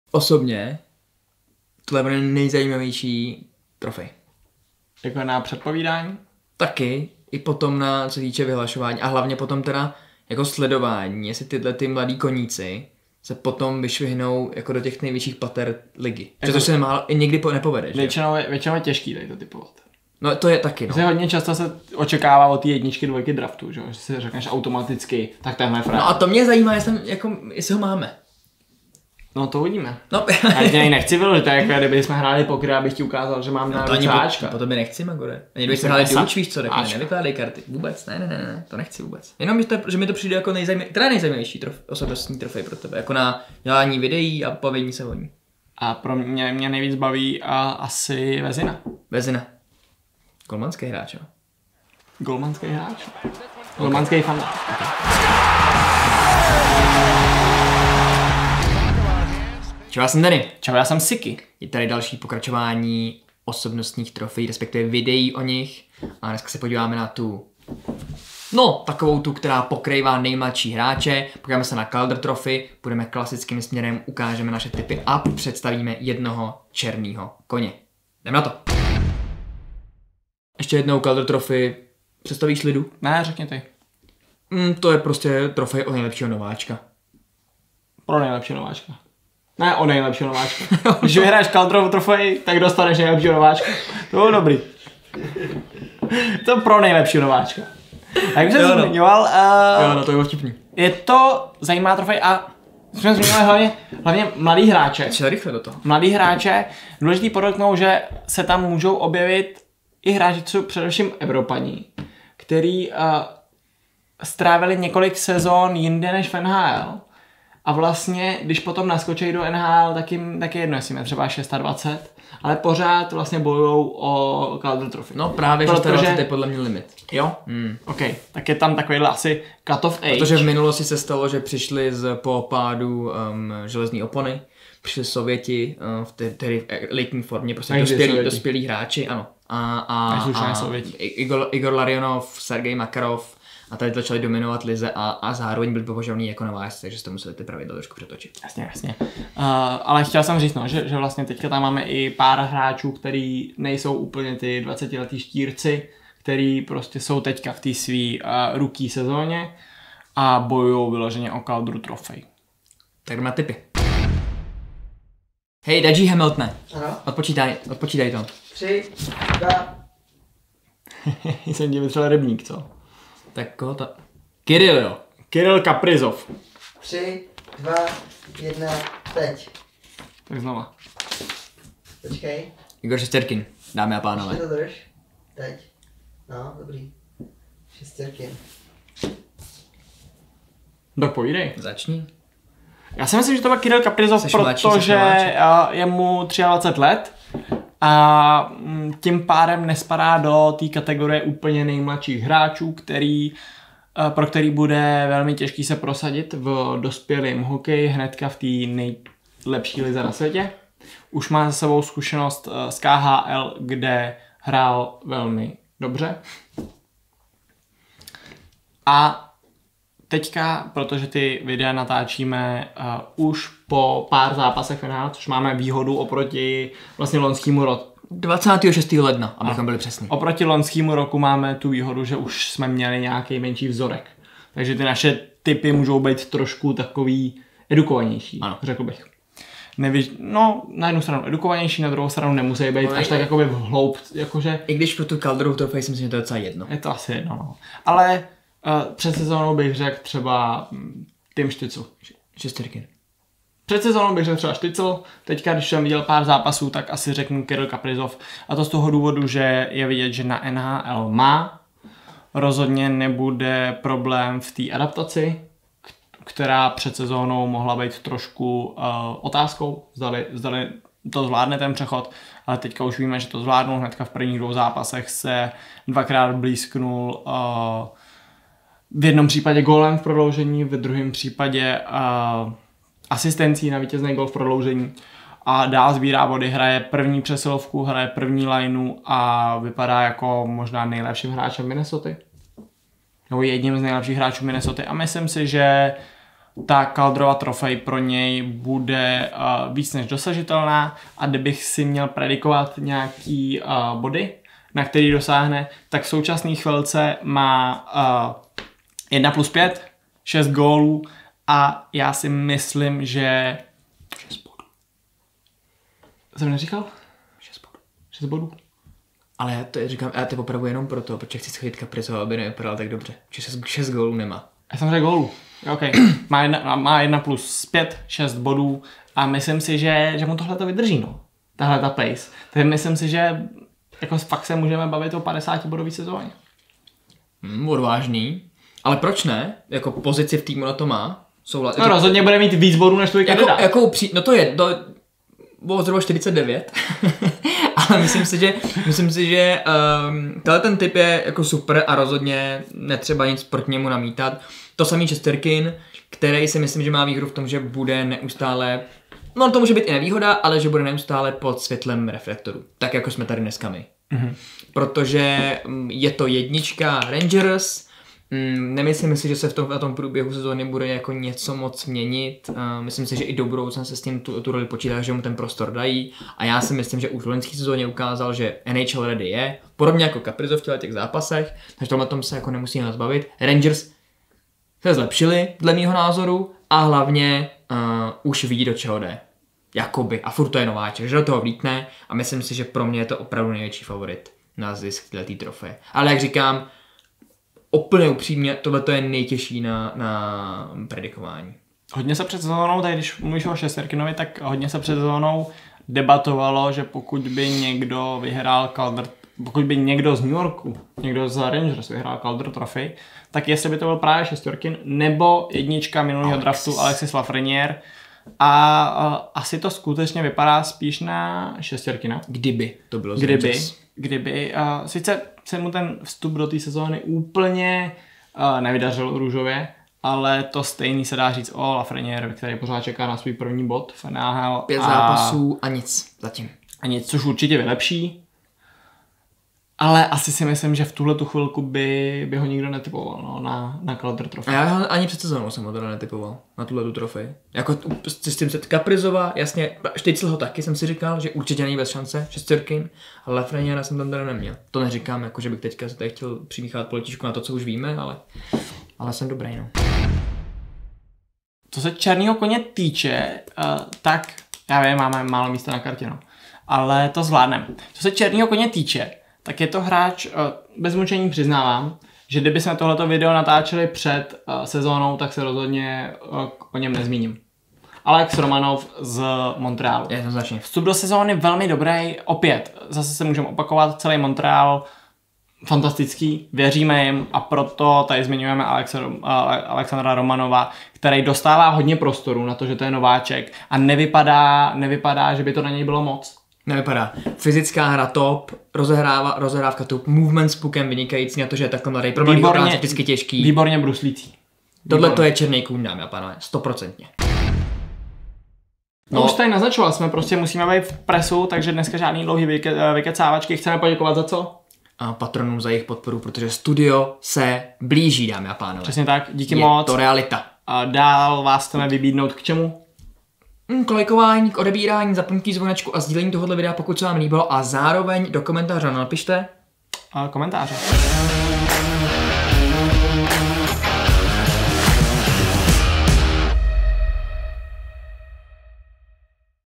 Tak mladej, tak skvělej! Kdo to letos bude? Calder Trophy! | Trophy Tracker #4 | 2020/2021
Calder Trophy každoročně získává nejlepší nováček uplynulé sezóny! Letos jsou zraky většiny upřeny směrem do New Yorku, kde o tuto trofej soutěží spoluhráči Shesterkin a Lafreniere. Bez šance určitě není Kaprizov z Minnesoty. Kdo je vaším favoritem a černým koněm?

A tohle čekni taky!!